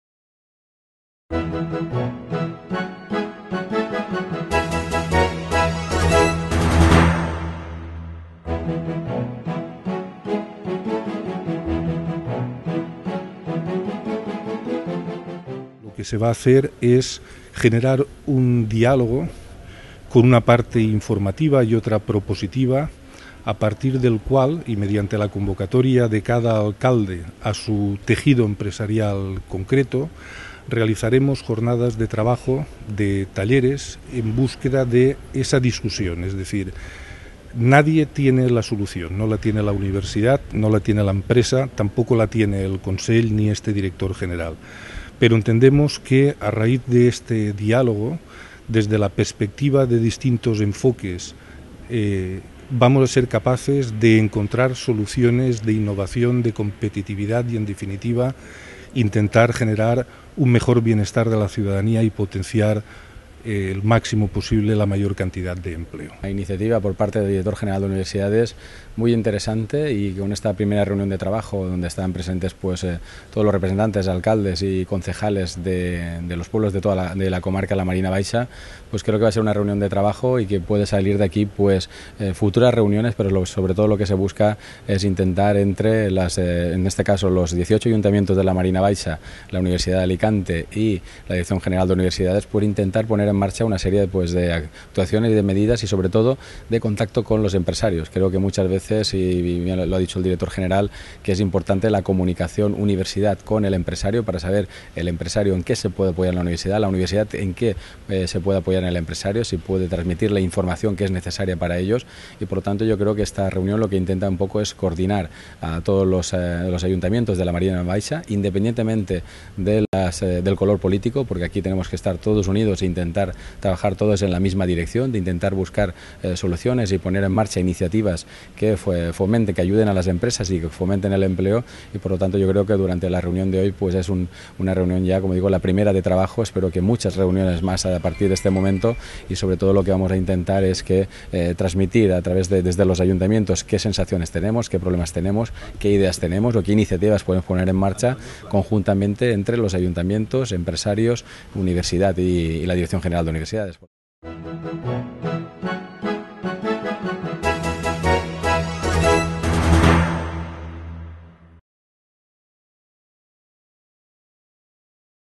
12/09/11 - Foro comarcal Empresa-Universidad para la creación de empleo. La Nucía.
Esta mañana en La Nucía se han reunido José Miguel Saval, director General de Universidades de la Conselleria de Educación junto con alcaldes de los 18 municipios de la Marina Baixa en una mesa de trabajo para la creación de un Foro "Empresas- Universidad" de la Marina Baixa para la creación de empleo. Bernabé Cano, alcalde de la Nucía destacó la importancia de "aunar esfuerzos y fomentar la colaboración multilateral (univ-empresas-aytos) para la creación de empleo".
El director general de Universidades de la Generalitat Valenciana, José Miguel Saval, destacó que se trata "un proyecto pionero en la Comunidad Valenciana y que cuenta con todo el apoyo de la Conselleria. Si fructifica y tiene éxito lo extrapolaremos al resto de comarcas de la Comunidad Valenciana. Iniciativas como esta multilaterales es lo que hace falta en épocas de crisis económica".
Esta reunión se ha celebrado en La Nucía porque es la sede  de la Universidad de Alicante en la Marina Baixa. Por parte de la Universidad ha estado presente Vicent Marines, como director de la Seu Univ. de La Nucía, además de José Miguel Saval del Director General de Universidad, Estudios Superiores y Ciencia de la Generalitat Valenciana y Bernabé Cano, alcalde de La Nucía han participado en esta mesa de trabajo Juan Bautista Saval alcalde de Callosa d'en Sarriá, Mª Ángeles Valdivieso concejala de Empleo de Benidorm, Jerónimo Lloret primer teniente alcalde de la Vila Joiosa, Maria France Berenguer concejala de Empleo de Polop, Paola Segura concejala de Turismo de Alfaz, Jaime Sellés primer teniente alcalde de Altea, Rafaela Llorens alcaldesa de Beniardá, David Blanes alcalde de Benifato, Felipe Miralles alcalde de Benimantell, Andrés Ferrer alcalde de Bolulla, Enrique Ponsoda alcalde de Castell de Guadalest, José Buades alcalde de Confrides,  Francisco Sánchez concejal de Empleo de Finestrat,  José Vicente Ferri alcalde de Orxeta, Santiago Cantó alcalde de Relleu y Natalia Hernández alcaldesa de Sella.

Plan de Acción Comarcal
Durante la reunión el Director General presentó el Plan de Acción Comarcal de Transferencia del Conocimiento y Competitividad para el Fomento de la Creación de Empleo. Un plan estratégico de futuro para la Marina Baixa.
El Plan pretende fomentar la coloración multilateral entre las administraciones locales (ayuntamientos), provinciales y autonómicas, las empresas (tejido productivo en todos sus segmentos) y la Universidad, para el fomento y creación de empleo. Para este objetivo es fundamental  la reactivación de la economía de los sectores productivos y crear las circunstancias idóneas y voluntad de asumir decisiones.
Para fomentar las circunstancias es necesario la transferencia de conocimiento entre las diferentes administraciones, el tejido productivo y la Universidad, que portaría I+D+I (Investigación+ Desarrollo+ Innovación).